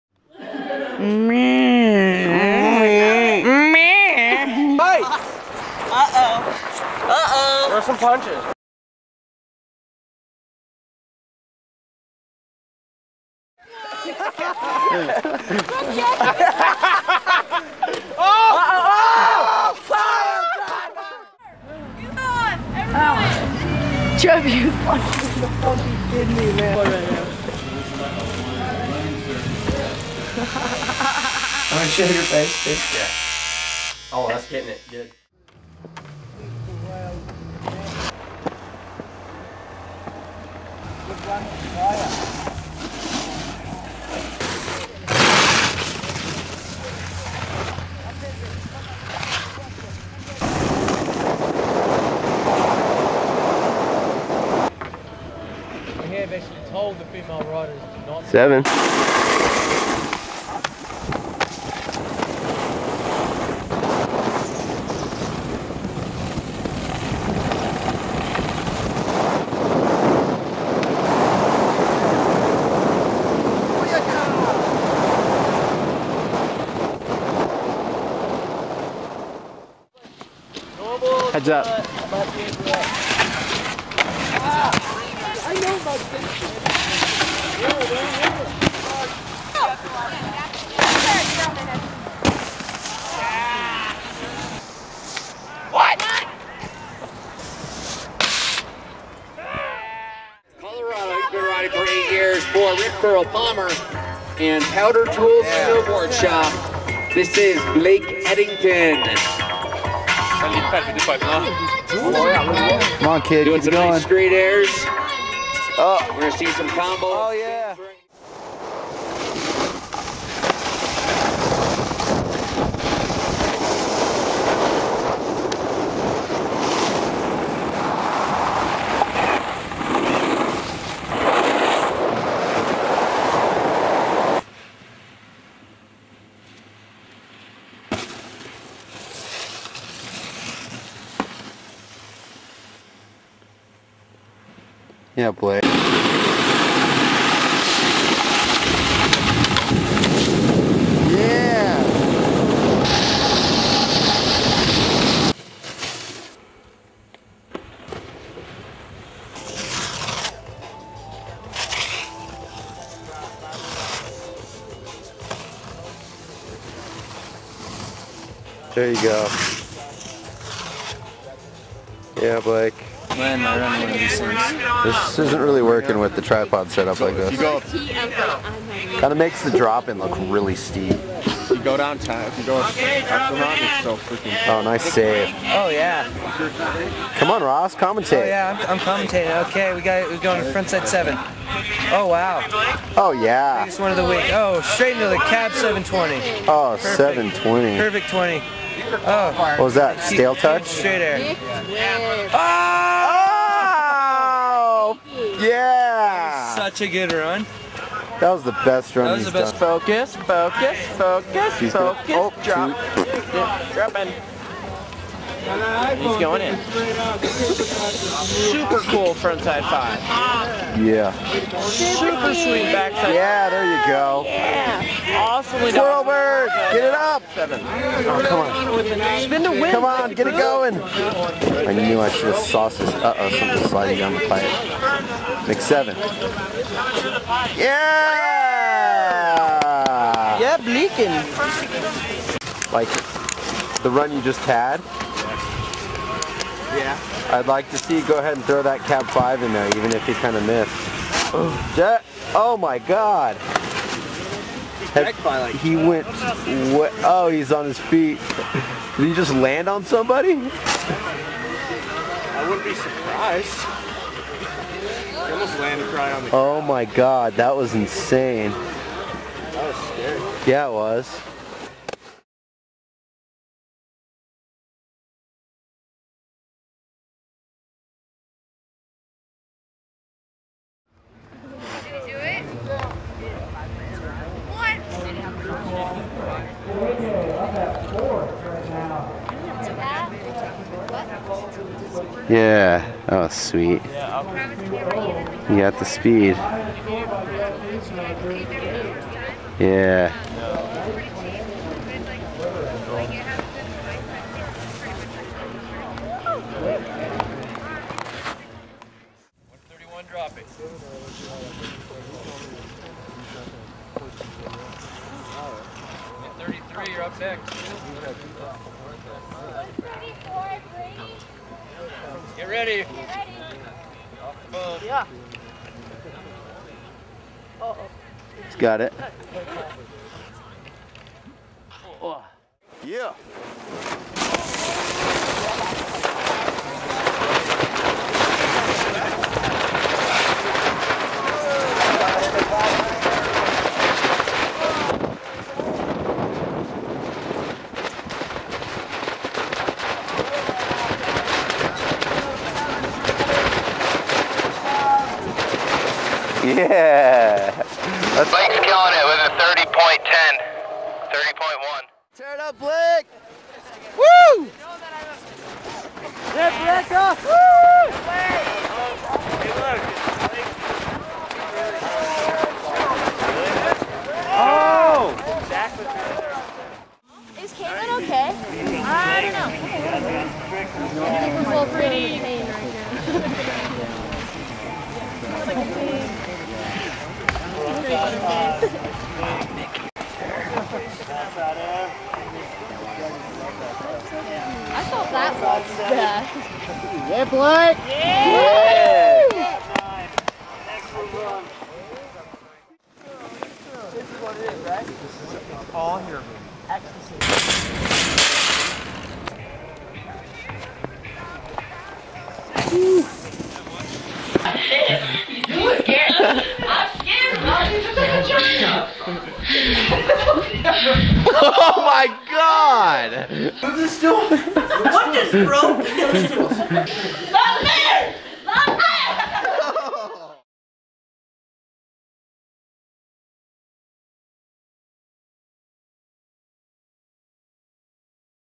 Blake Eddington Snowboarding Around
Blake Eddington rides from New Zealand all the way to the East Coast as he travels with the Steamboat Springs Winter Sports Clubs Pro-Am Freestyle Snowboarding Team